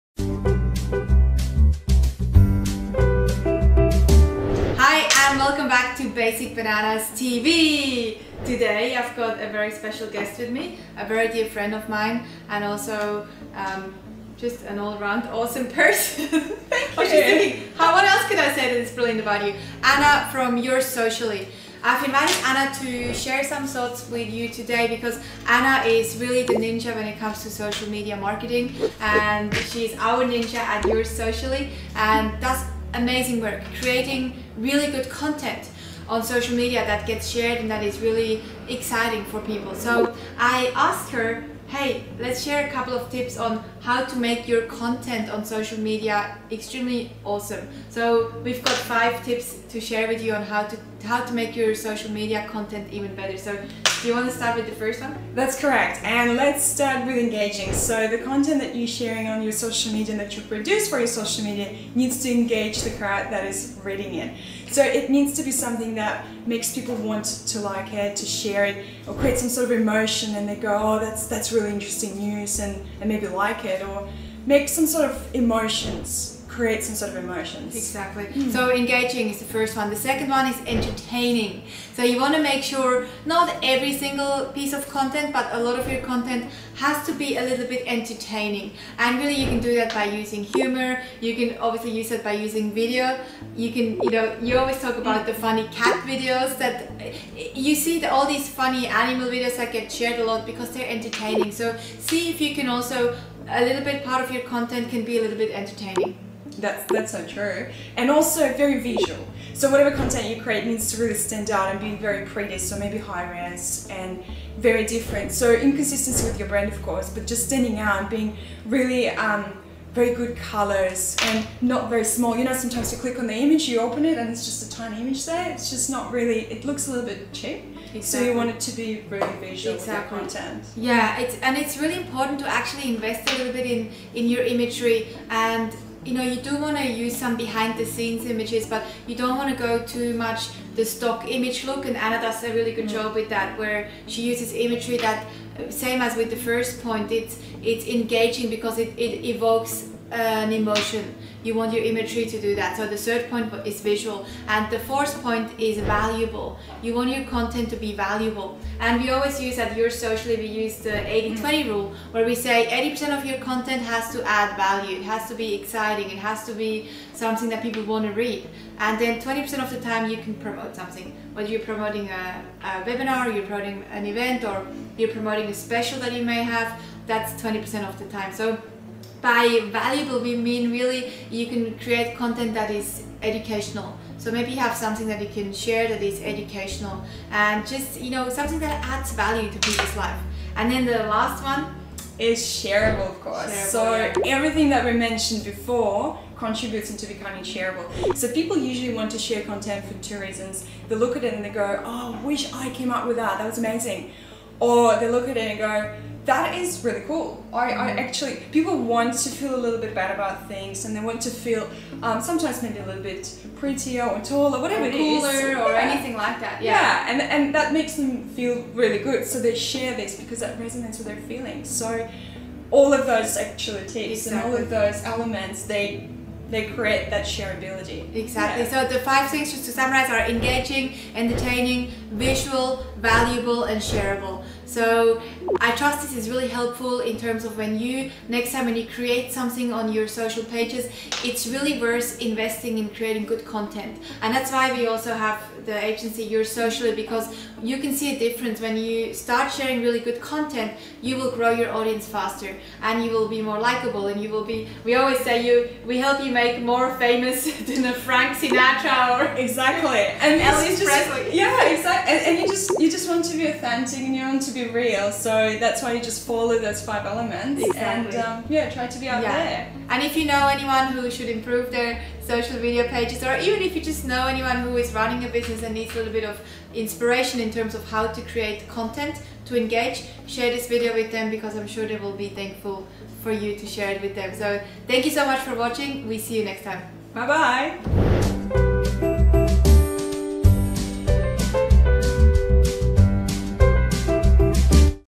Social Media Tip: How to create content that gets shared
for more valuable marketing tips. We love spoiling our business community!

In this episode of Basic Bananas TV, Franziska and Anna from YoursSocially.com.au discuss five ways to make your social media content more engaging and shareable.

1:05 How to engage your audience
2:31s What type of imagery to use
3:19s What makes your content shareable

As always, thank you for being part of this amazing business tribe! We truly appreciate you!